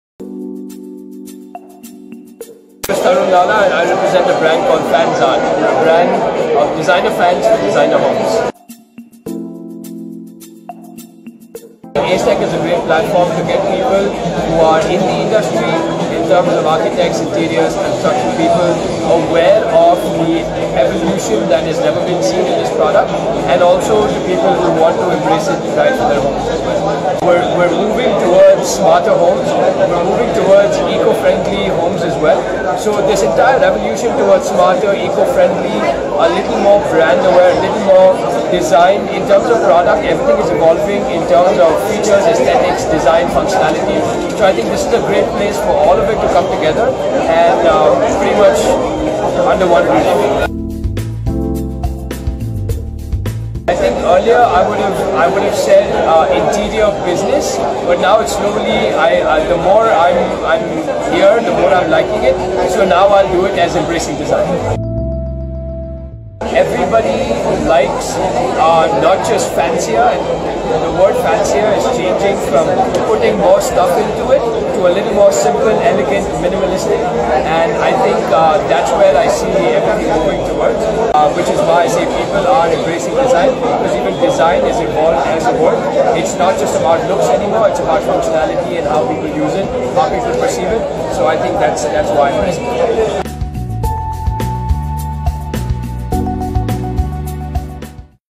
Exhibitor Byte
'Tarun Lala', the representative for Fanzart had quite an interesting take on ACETECH and it's progress in BanayeBehetarBharat